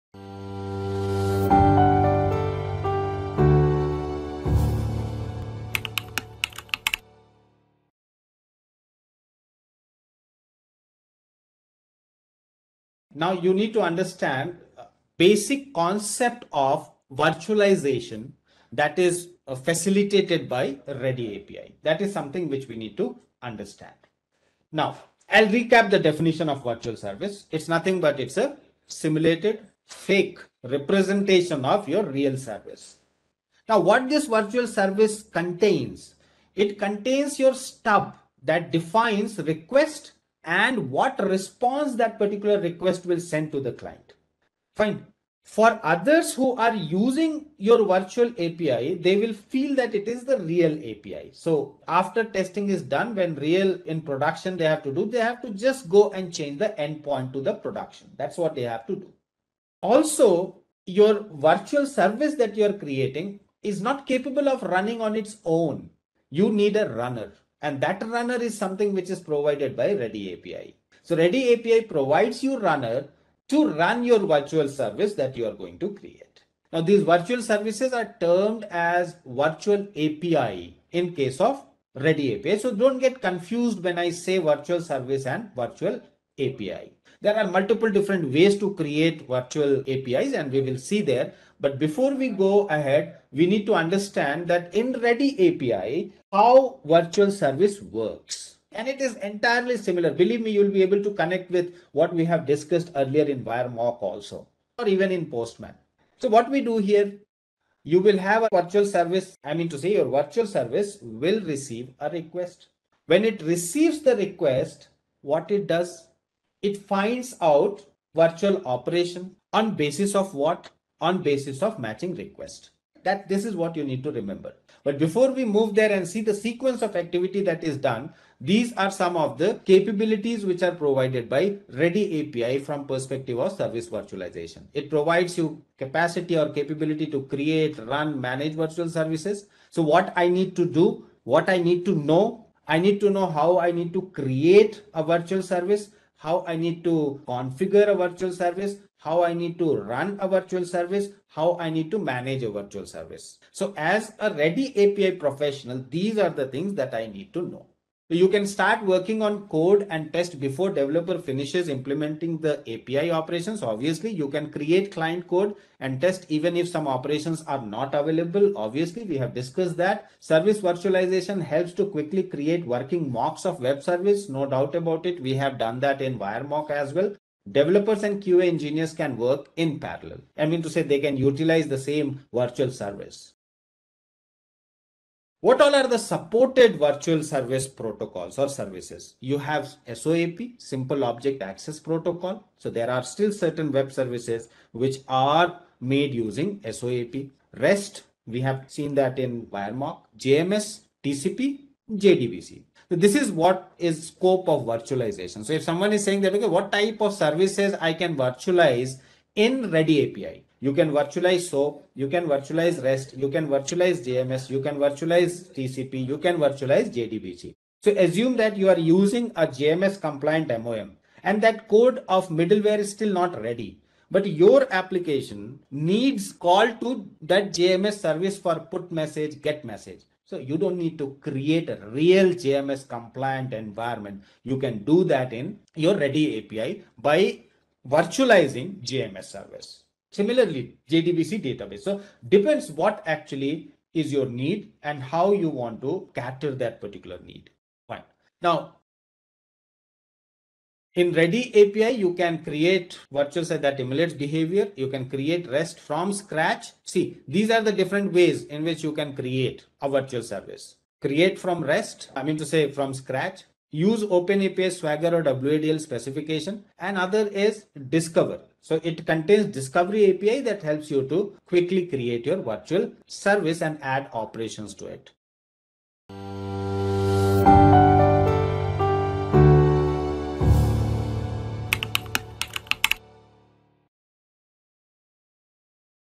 Service Virtualization using ReadyAPI
In this video I'm going to show you how ReadyAPI gives you a sandboxed environment for testing services and APIs. You can even use it to test your own APIs, simulate changes in realtime, and get instant responses just like you would in a live production environment. ReadyAPI is the fastest way to make changes and test your application before you go live.